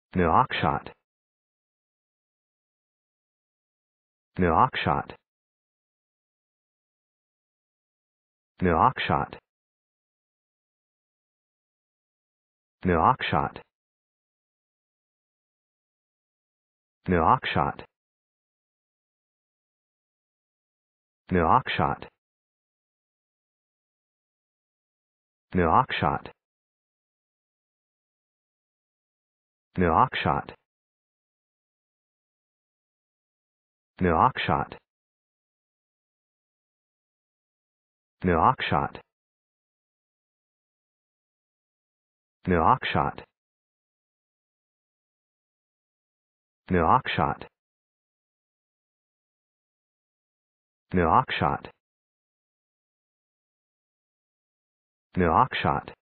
How to pronounce Nouakchott | English pronunciation
Learn how to pronounce Nouakchott in English
NOUAKCHOTT
Pronunciation of Nouakchott:

Definition of Nouakchott: (noun) capital of Mauritania; located in western Mauritania near the Atlantic coast